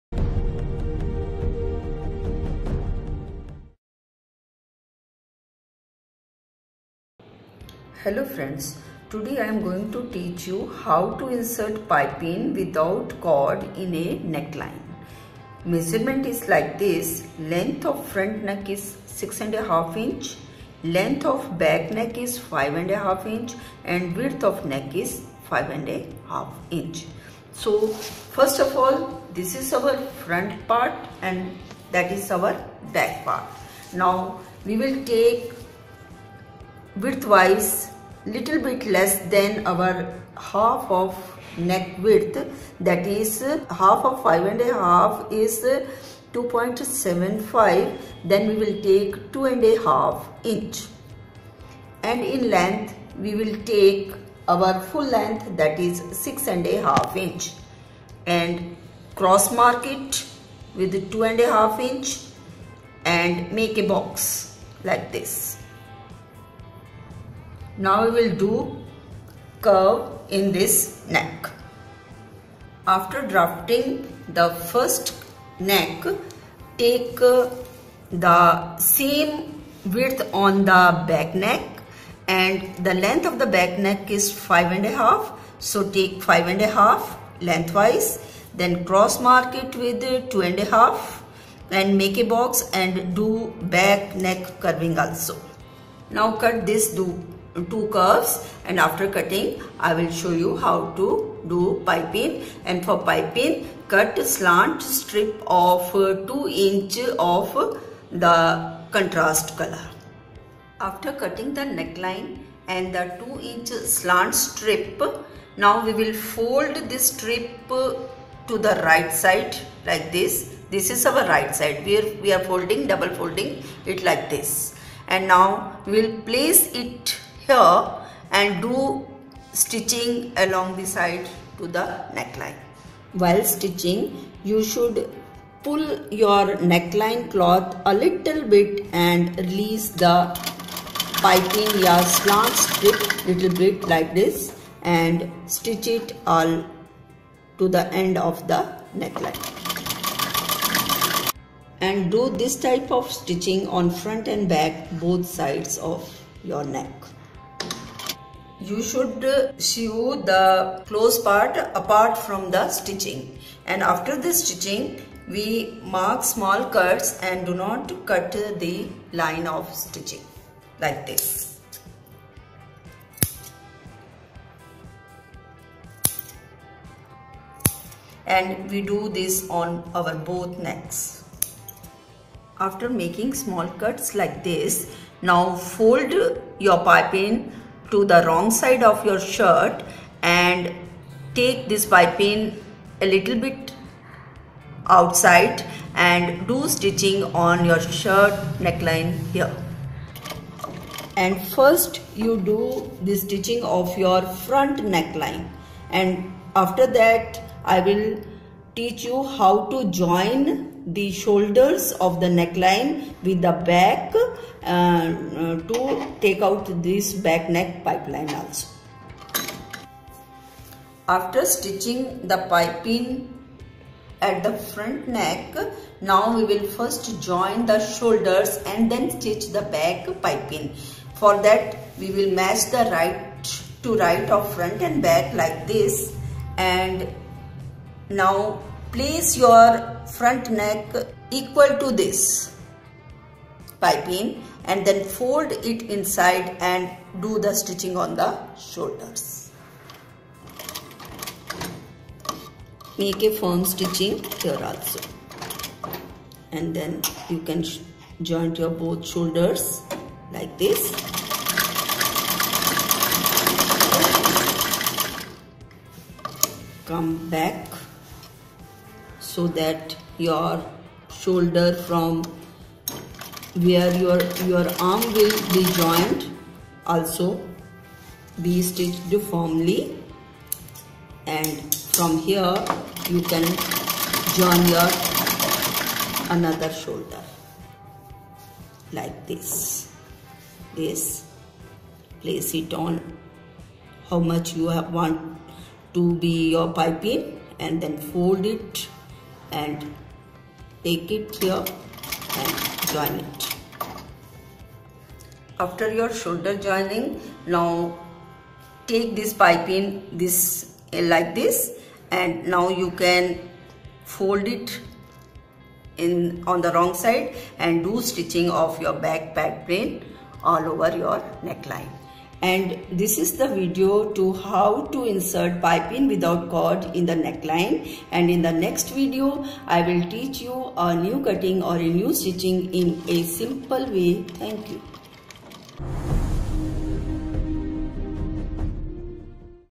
How To Sew Piping Without Cord In Neckline | English - Tailoring With Usha
Learn the simplest way of sewing piping without cord in a neckline with "Tailoring With Usha". Full video along with the making of piping without cord and how to do cutting and stitching for the perfect fit.